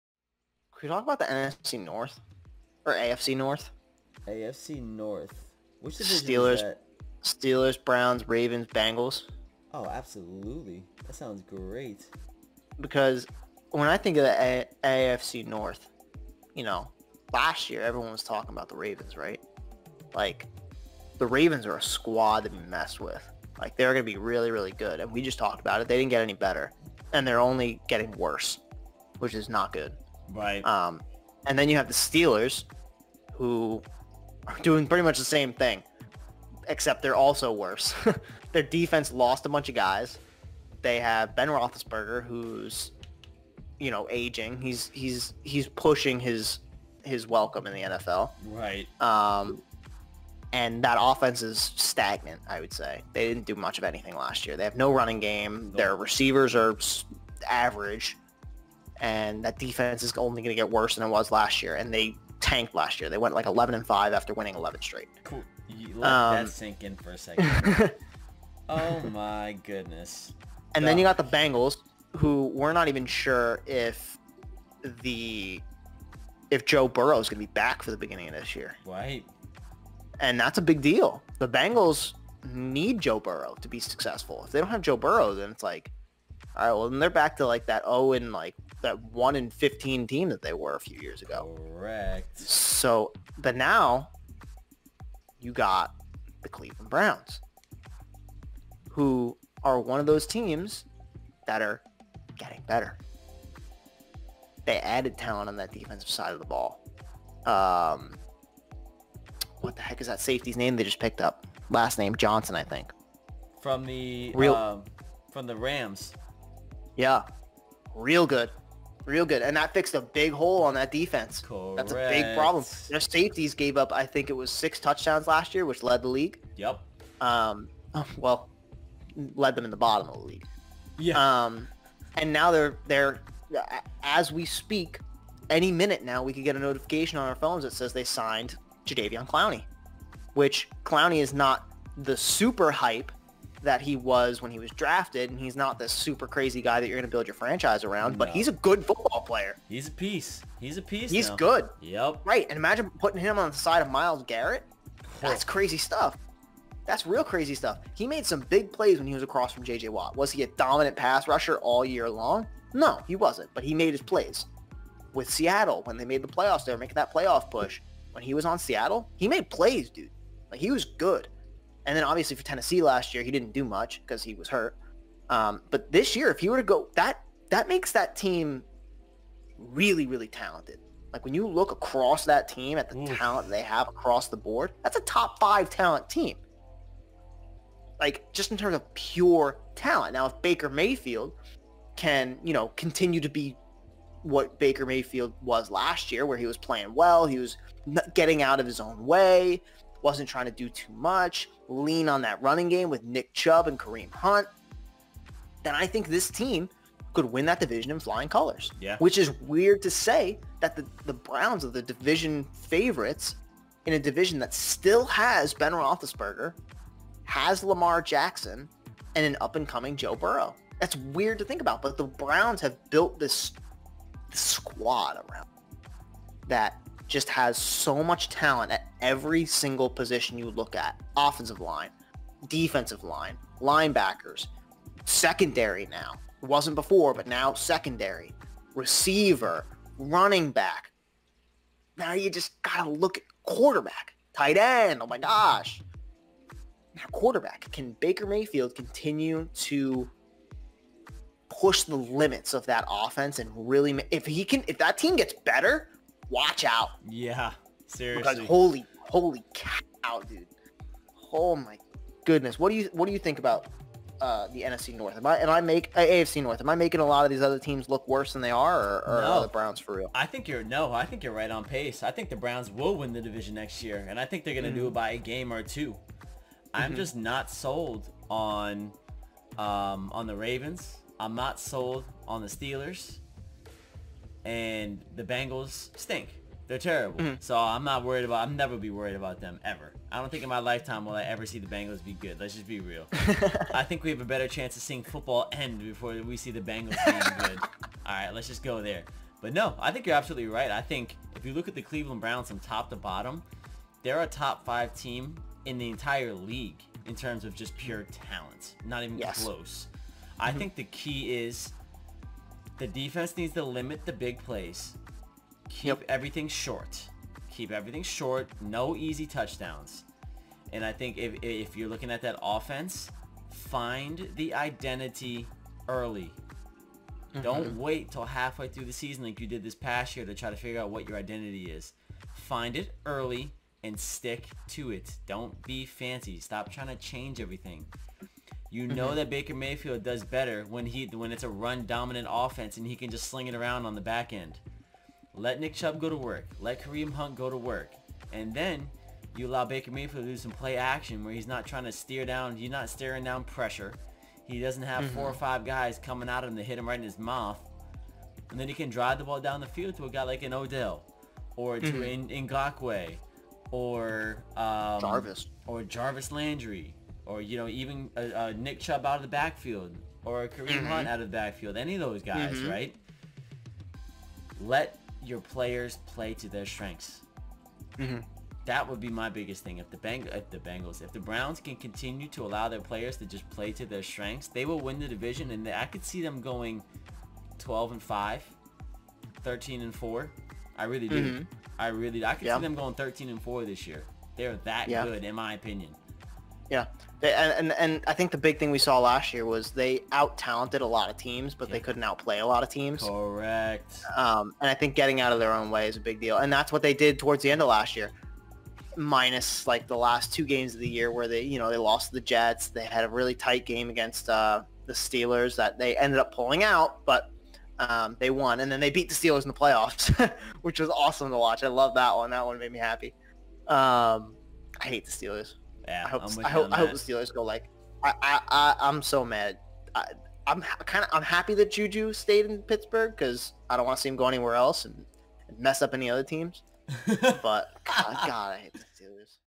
The Sit Down: Sports Edition. The AFC North! Who we think will WIN the division!
The Sit Down: Sports Edition. The AFC North! Who we think will WIN the division! Andrew and I share our thoughts on the AFC NORTH conference as well as who we believe will win the conference and why!